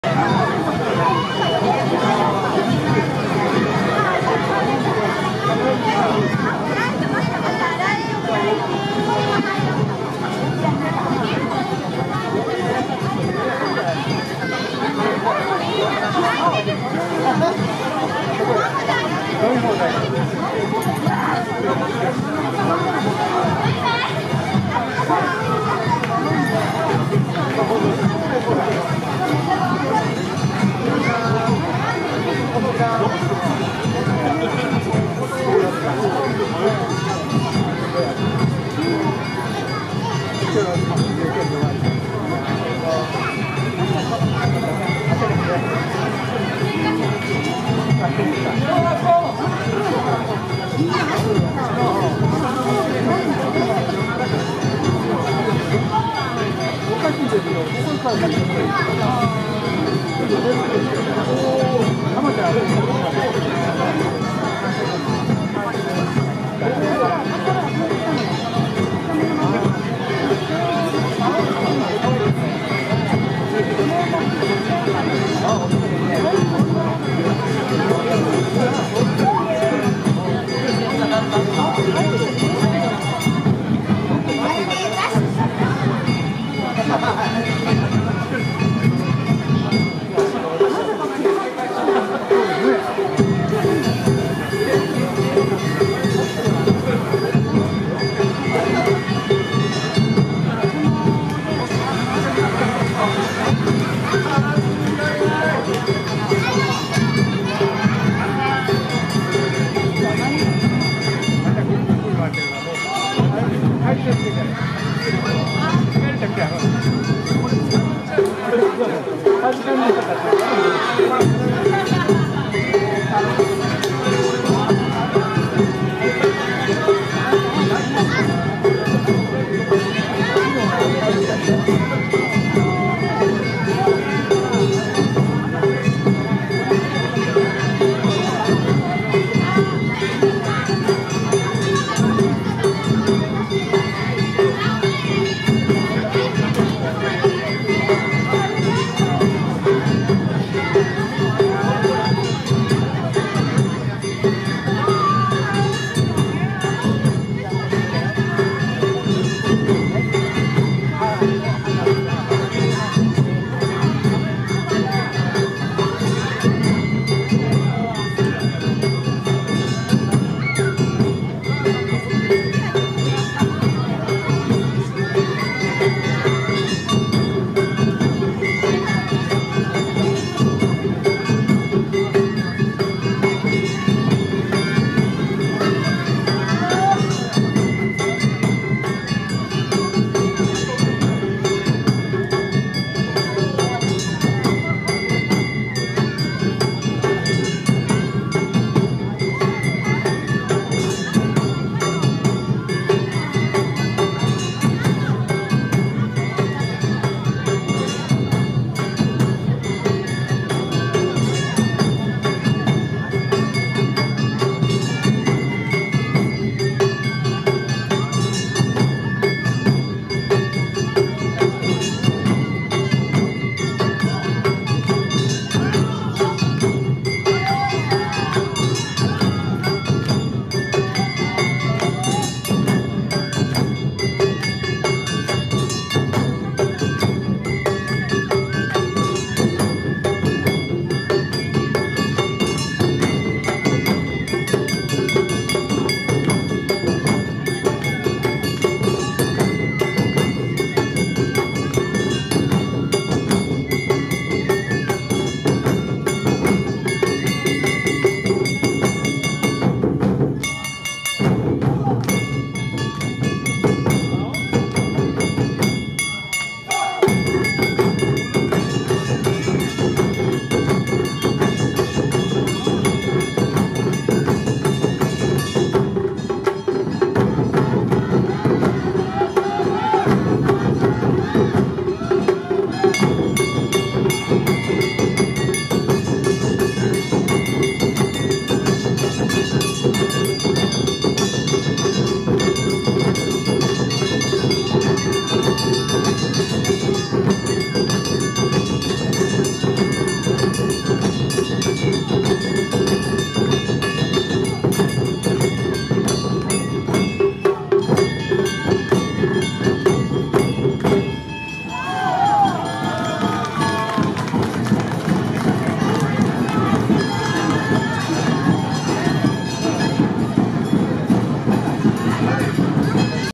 目黒銀座連（2巡目）／第39回 かせい阿波おどり2018
2018./07/29
【会場】都立家政駅周辺
【主催】都立家政商店街振興組合